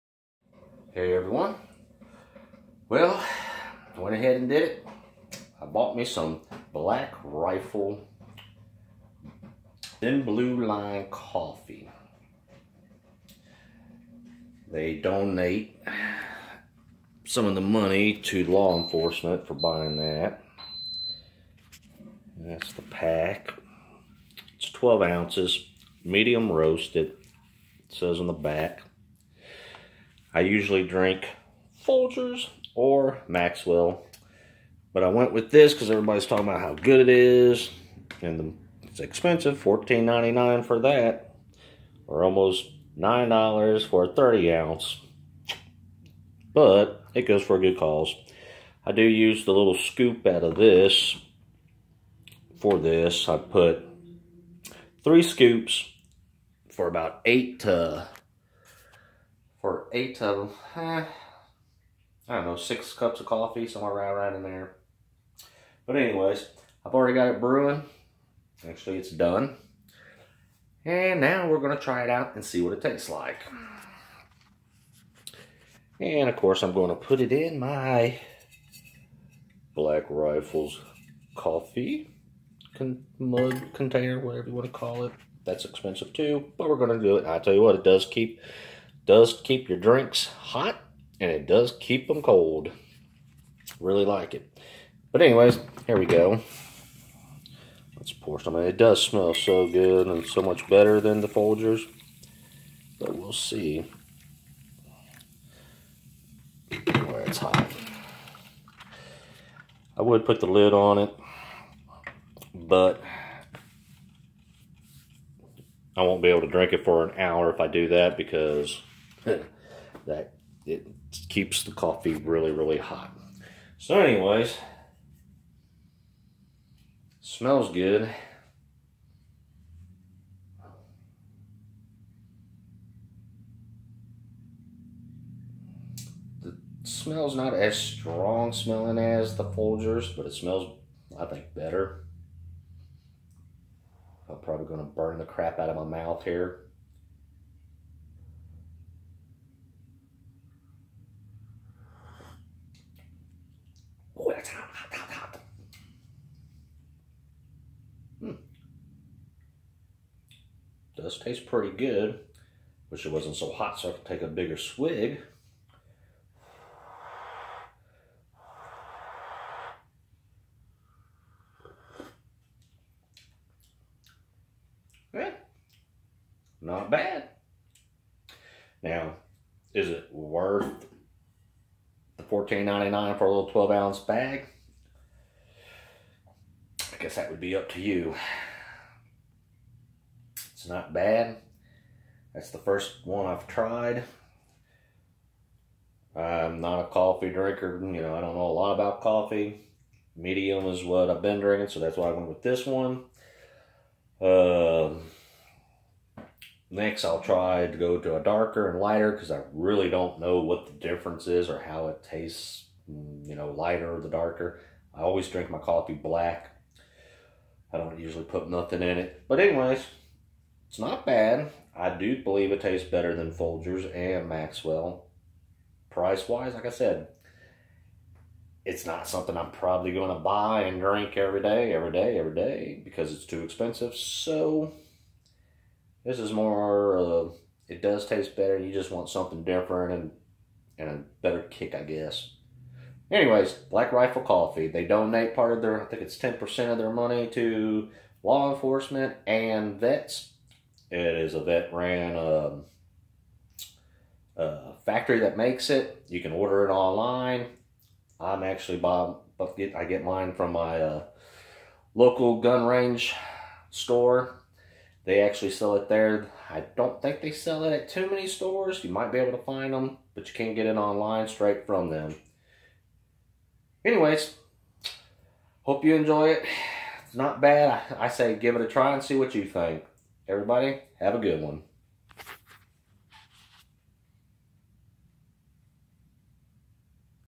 Black Rifle Coffee
Trying Black rifle coffee for the first time.what do i think.